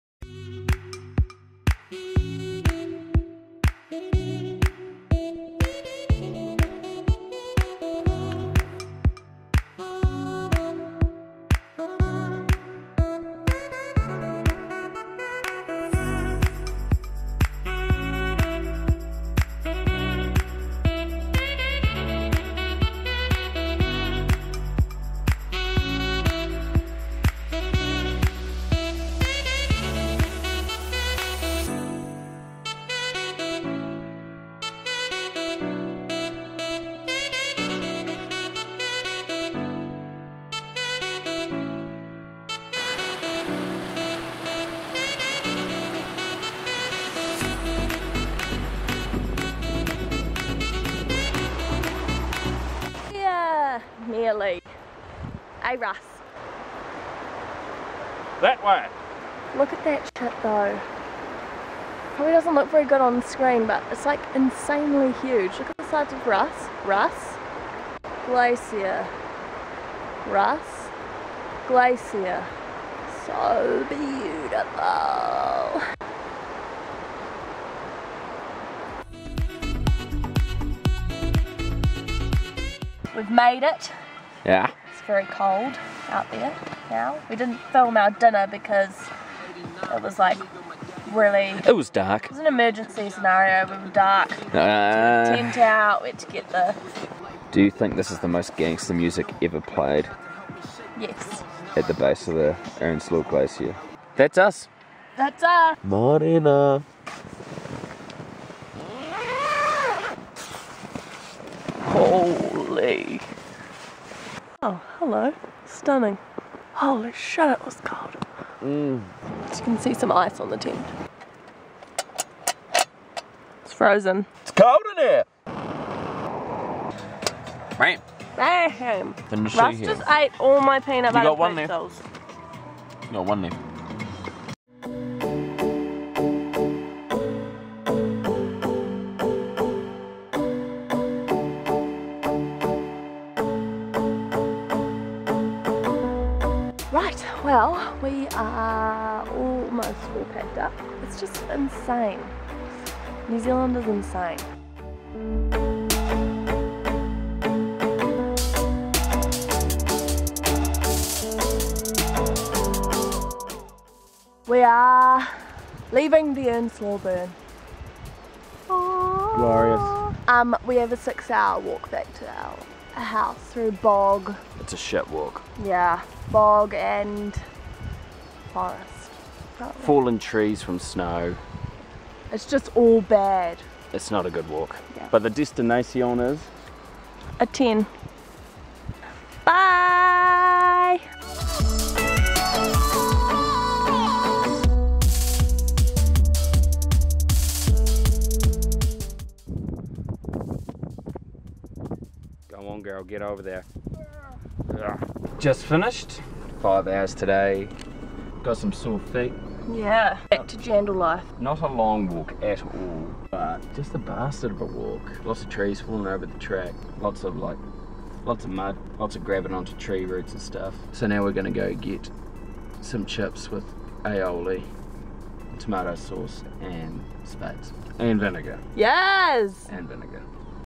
Earnslaw Burn, Queenstown NZ
We took a little stroll around our backyard In Queenstown.